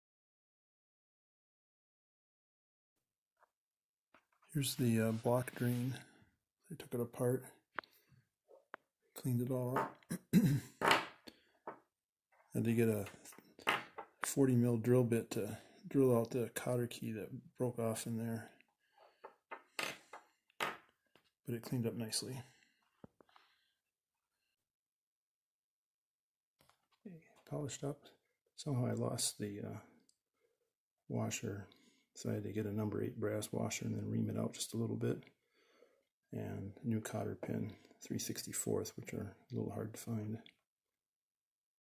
1959 Triumph TR3, Part 27, Engine Coolant Drain Valve
Just a short video showing drain valve apart, cleaned up and then put back together.